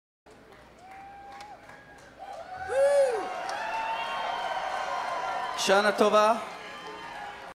Jeff Tweedy of Wilco wishes his crowd a Shana Tova (Happy Jewish New Year)
Jeff Tweedy of Wilco wishes the crowd a Happy Jewish New Year in Hebrew during the band's concert in Columbia, MO on 9/16/2012.